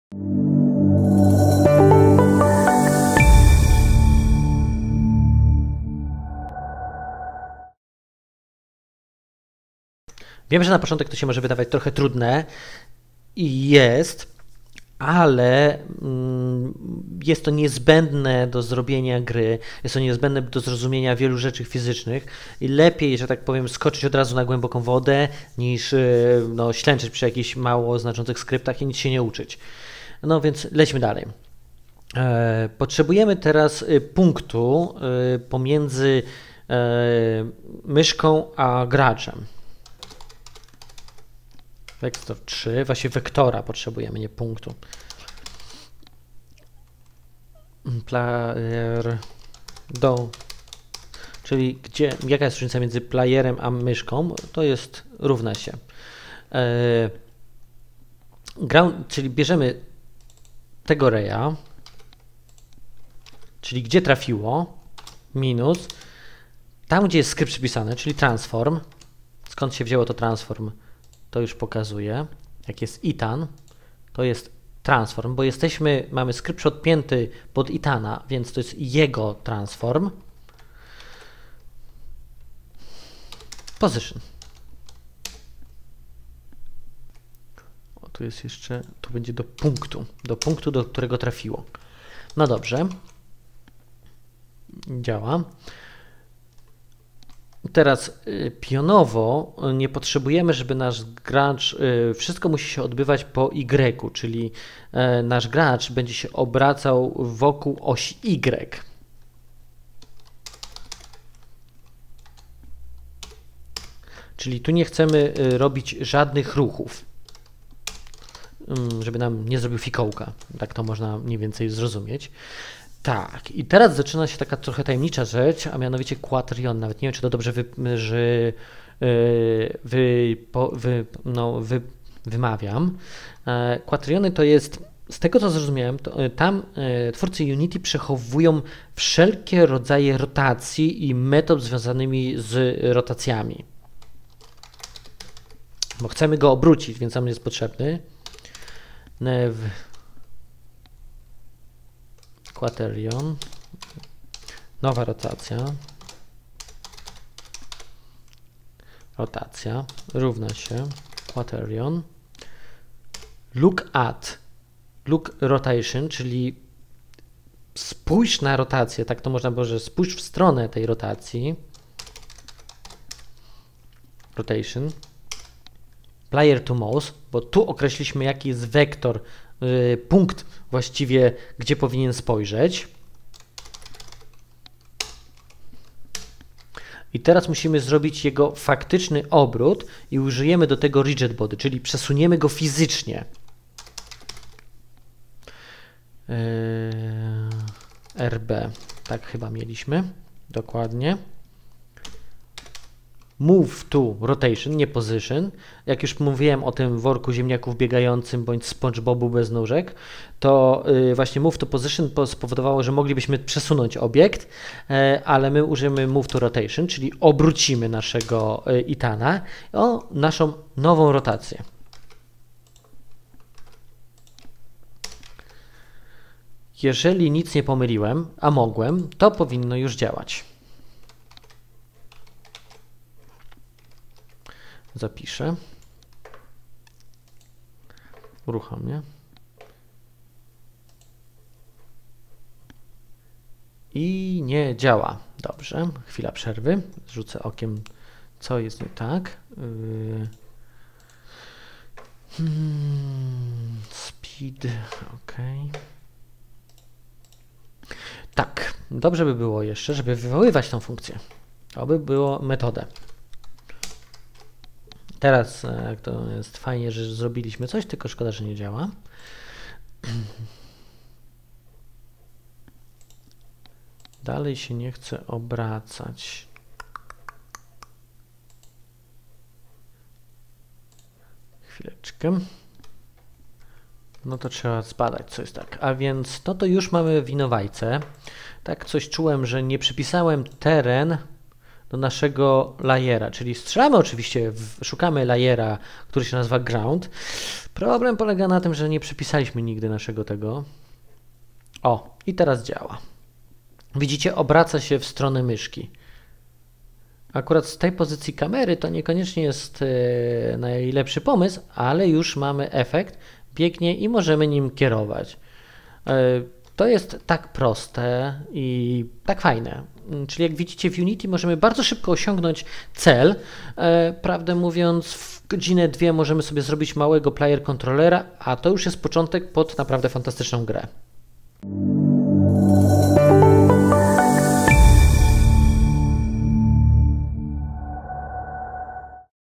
Podstawy Unity 3D - Lekcja 09 - Animowanie bohatera (część 8)
Kurs "Podstawy Unity 3D" został przygotowany z myślą o wszystkich którzy chcą rozpocząć przygodę z Unity. Teraz krok po kroku stworzymy zarówno animacje naszego bohatera jak i kontroler pozwalający nam na jego poruszanie.